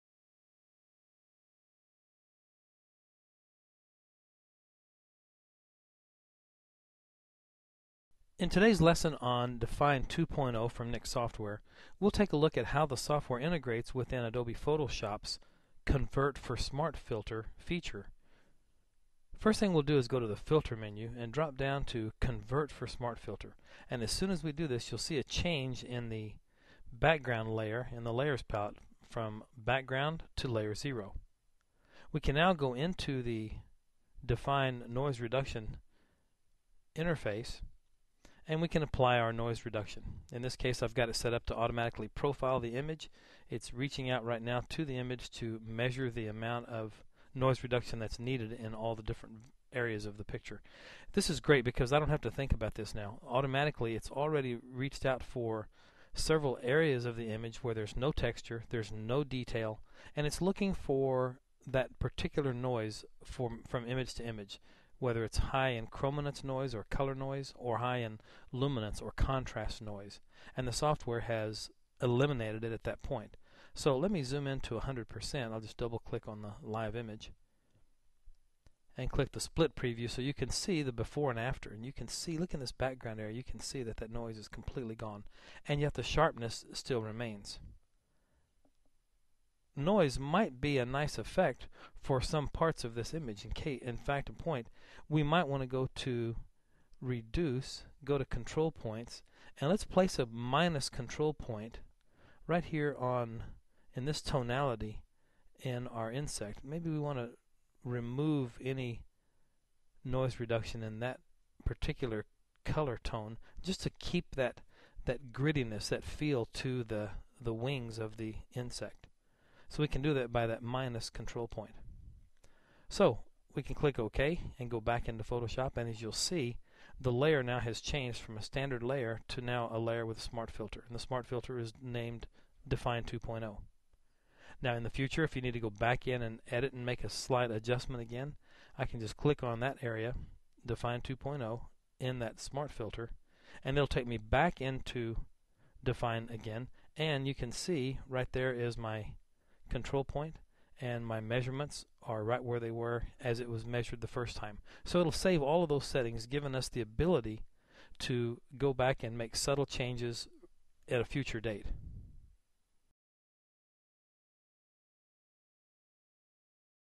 Nik Software original training -- Dfine Smart Filter Compatibility
Get an introduction to the new Dfine 2.0 interface and find out how Dfine 2.0 has been designed to provide a more productive workflow.